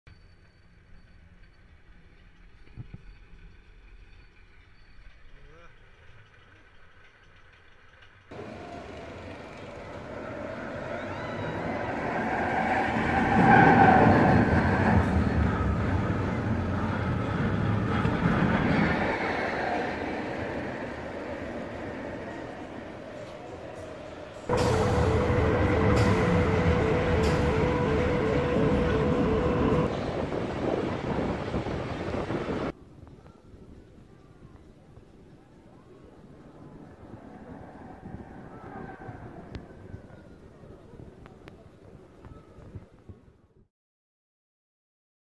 Camel Roller Coaster Jetcoaster at Nasu Highland Park
This is one of the signature rides at Nasu Highland park, a long jetcoaster built by Meisho Amusement Machines. Despite the camel name, it doesnt actually have many camel back elements or airtime hills!